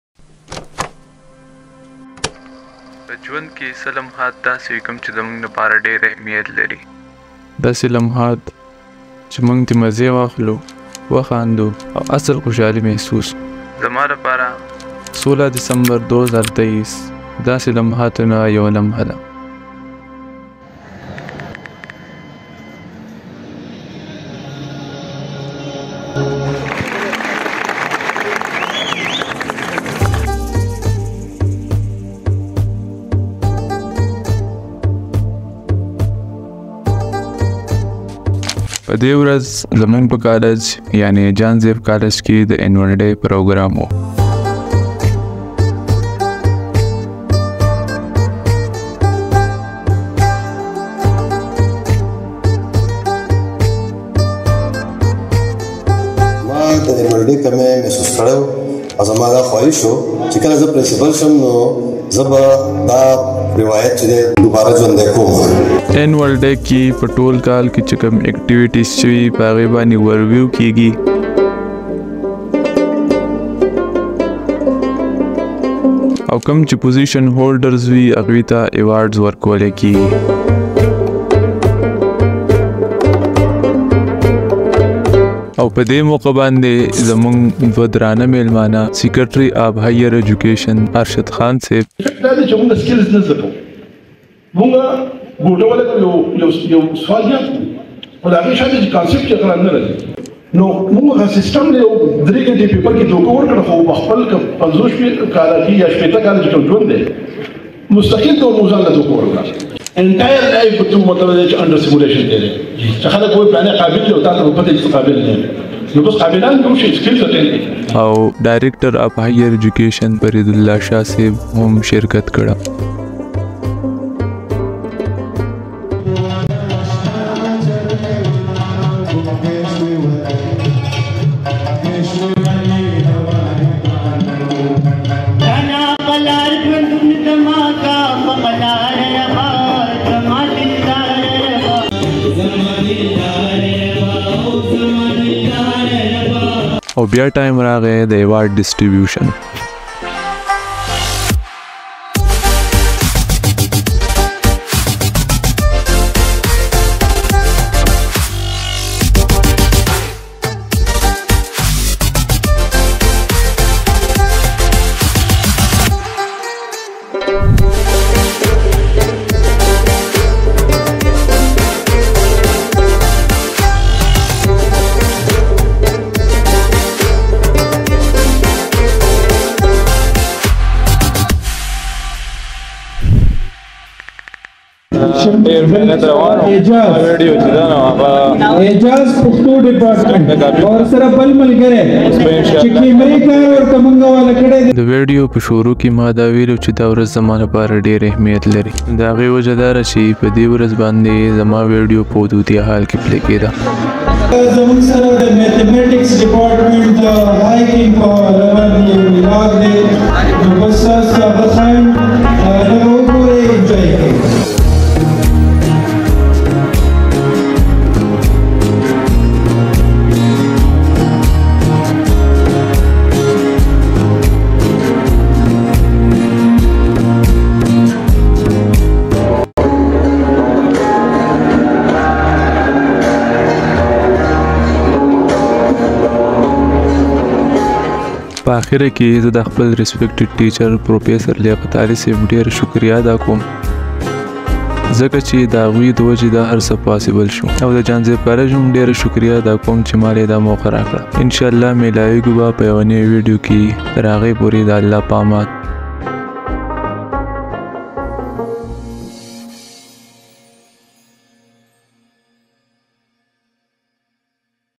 Annual Day At Jahanzeb College Swat | Short documentary by TalhaKhan | Music
Hi,my name is Talha and from Swat KPK Pakistan.this Video is about an annual day celebration at Govt Post Graduate Jahanzeb college Saidu Sharif Swat.the video is in Pashto language and unfortunately YouTube have not added the Pashto language to automate English subtitles,so if you watch the video and you like my work then insha'Allah with time I'll be uploading in Urdu and English languages..